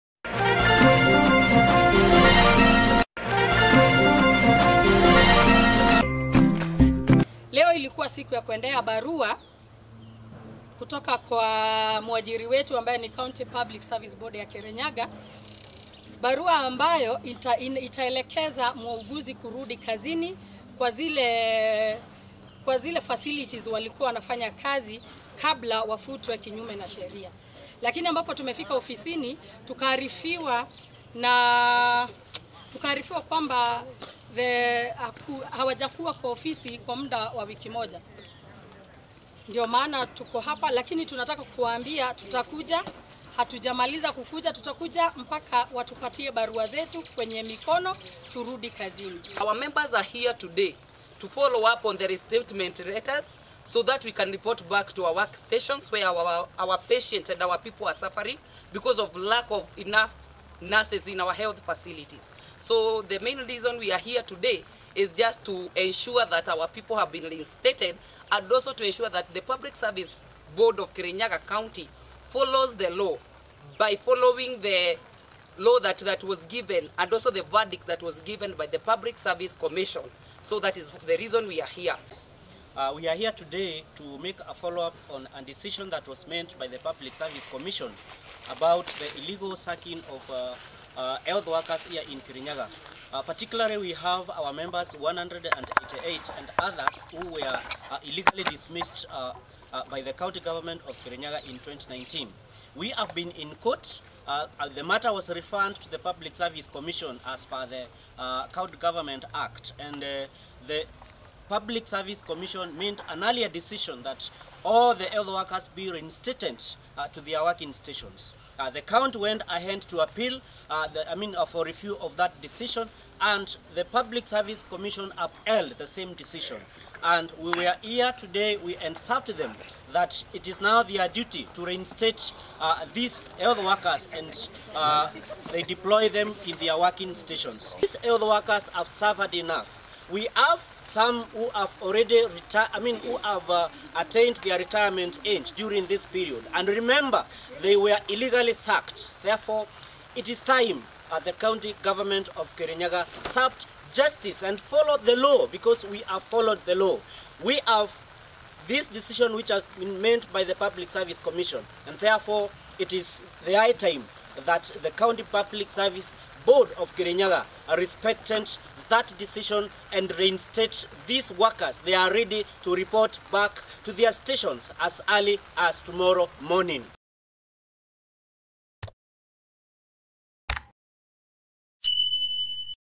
STRESS IS KILLING THEM: FIRED KIRINYAGA NURSES WANT THE COUNTY GOVERNMENT TO REINSTATE THEM ASAP
More than 188 sacked Kirinyaga nurses have demanded Kirinyaga County Public Service Board to reinstate them as directed by the Public Service Commission(PSC).
Led by the Kenya National Union of Nurses (KNUN)Joseph Ngwasi, the medic said the county has totally ignored the PSC directives which authorized it to absorb them back.
The health officers claim that some of them are approaching retirement age and are still not in service.
Ngwasi said the county has no legal right to fire them and replace them with workers under contract terms yet they were permanent workers with pensionable terms.
Lucy Mburu, Assistant general secretary KNUN, said they are ready to report to the work immediately. She added the county should follow the law and PSC verdict.
The health officers downed their tools in 2019 over deplorable working conditions and filth at Kerugoya Referral Hospital. New workers were hired to take over their jobs.